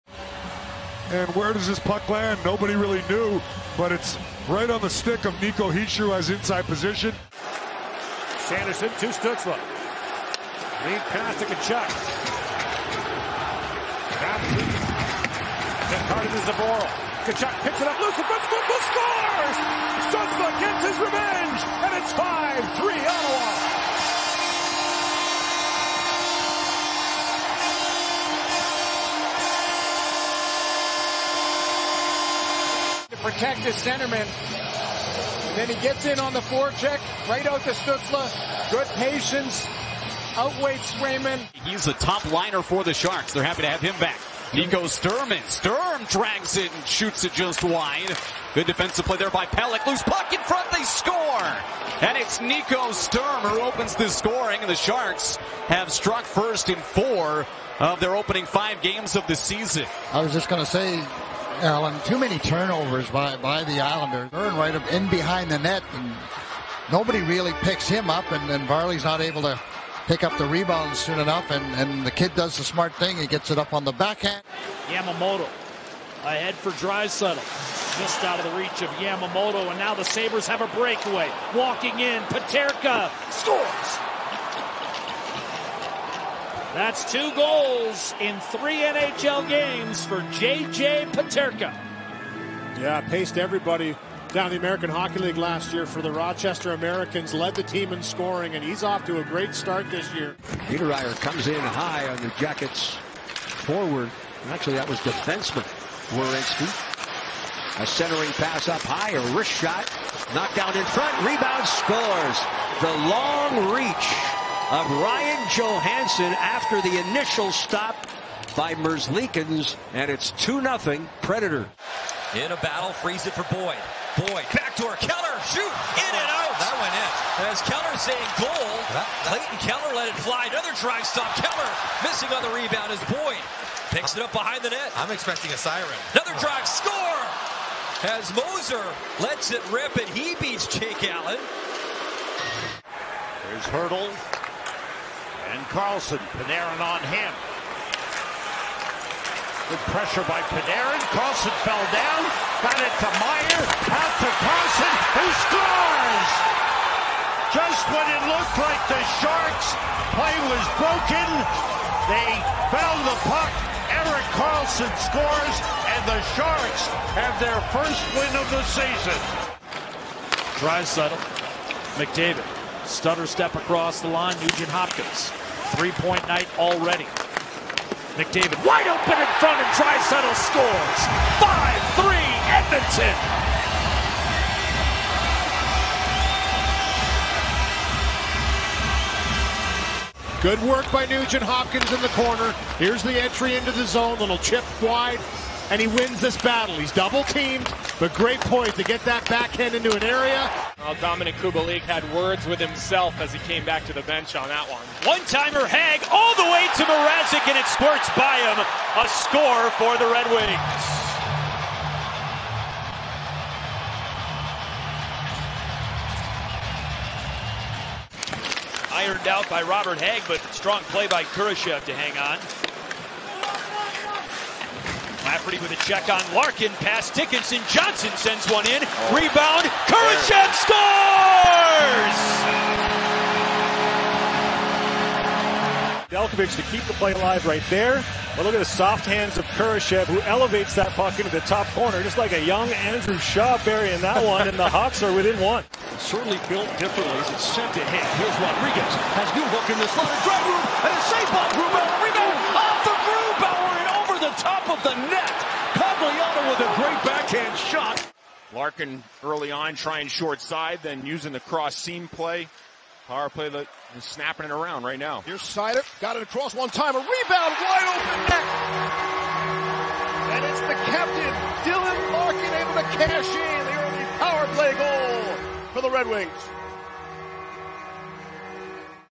Die DACH-Highlights der Woche mit Nico Hischier, J.J. Peterka und Timo Meier
Sieh dir die Highlights der Woche der DACH-Spieler an, mit Szenen von Nico Hischier, J.J. Peterka und Timo Meier

Feeling social?